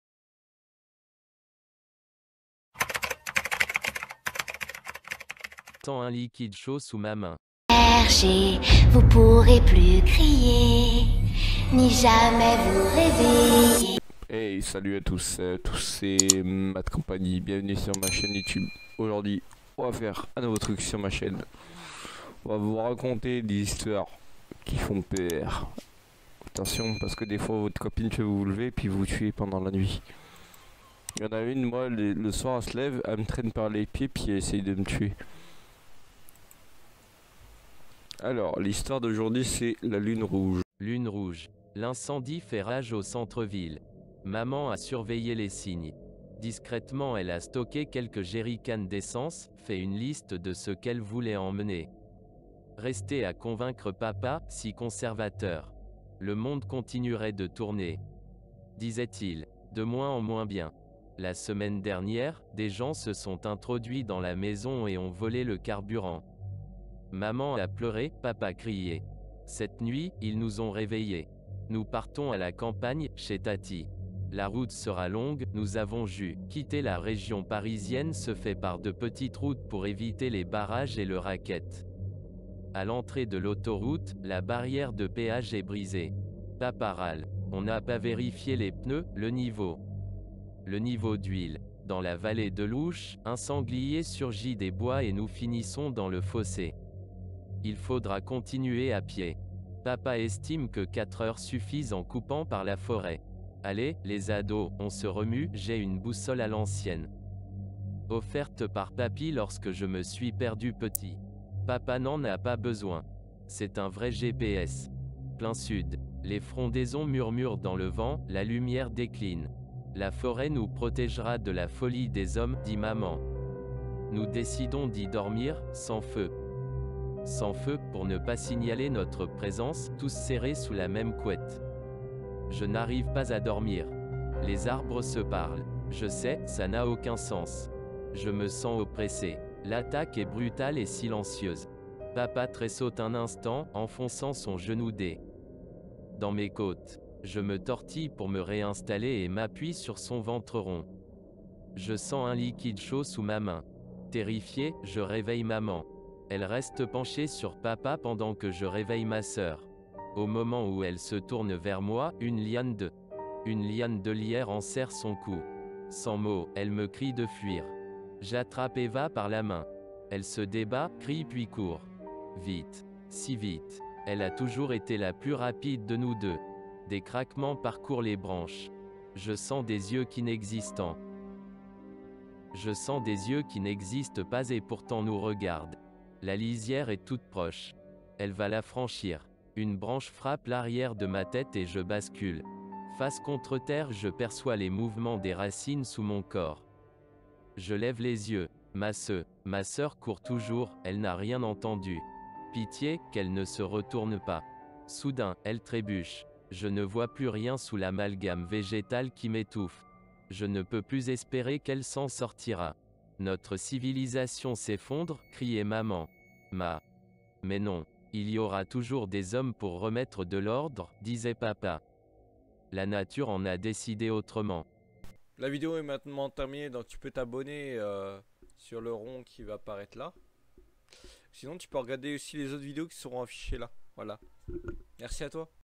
la lune rouge histoire d'horreur
voila une nouvelle série d'histoire d'horreur courte
je suis pas trop fore en animation animé dsl

voila le texte

Lune rouge. L’incendie fait rage au centre-ville.
Maman a surveillé les signes. Discrètement elle a stocké quelques jerricanes d’essence, fait une liste de ce qu’elle voulait emmener. Rester à convaincre papa, si conservateur. « Le monde continuerait de tourner », disait-il. De moins en moins bien. La semaine dernière, des gens se sont introduit dans la maison et ont volé le carburant. Maman a pleuré, papa crié.
Cette nuit, ils nous ont réveillés. Nous partons à la campagne, chez tatie. La route sera longue, nous avons juste assez d’essence. Quitter la région parisienne se fait par de petites routes pour éviter les barrages et le racket. À l’entrée de l’autoroute, la barrière de péage est brisée.
Papa râle. On n’a pas vérifié les pneus, le niveau d’huile. Dans la vallée de l’Ouche, un sanglier surgit des bois et nous finissons dans le fossé. Il faudra continuer à pied. Papa estime que quatre heures suffisent en coupant par la forêt. « Allez, les ados, on se remue »
J’ai une boussole à l’ancienne, offerte par papi lorsque je me suis perdu petit. Papa n’en a pas besoin. C’est un vrai GPS. Plein sud.
Les frondaisons murmurent dans le vent ; la lumière décline. « La forêt nous protégera de la folie des hommes » dit maman. Nous décidons d’y dormir, sans feu, pour ne pas signaler notre présence, tous serrés sous la même couette.
Je n’arrive pas à dormir. Les arbres se parlent. Je sais, ça n’a aucun sens. Je me sens oppressé. L’attaque est brutale et silencieuse. Papa tressaute un instant, enfonçant son genou dans mes côtes. Je me tortille pour me réinstaller et m’appuie sur son ventre rond. Je sens un liquide chaud sous ma main.
Terrifié, je réveille maman. Elle reste penchée sur papa pendant que je réveille ma sœur. Au moment où elle se tourne vers moi, une liane de lierre enserre son cou. Sans mot, elle me crie de fuir. J’attrape Eva par la main. Elle se débat, crie puis cours. Vite. Si vite. Elle a toujours été la plus rapide de nous deux. Des craquements parcourent les branches. Je sens des yeux qui n’existent pas et pourtant nous regardent.
La lisière est toute proche. Elle va la franchir. Une branche frappe l’arrière de ma tête et je bascule. Face contre terre je perçois les mouvements des racines sous mon corps. Je lève les yeux. Ma sœur court toujours, elle n’a rien entendu. Pitié, qu’elle ne se retourne pas. Soudain, elle trébuche. Je ne vois plus rien sous l’amalgame végétal qui m’étouffe. Je ne peux plus espérer qu’elle s’en sortira.
« Notre civilisation s’effondre » criait maman. « Mais non. Il y aura toujours des hommes pour remettre de l’ordre » disait papa.
La nature en a décidé autrement.